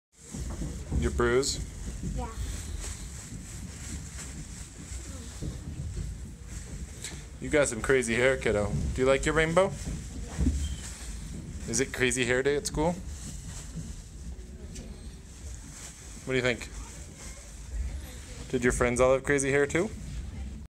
Crazy Hair Day
Aeryn wanted a "rainbow" for crazy hair day. She loved it, and she got a lot of compliments.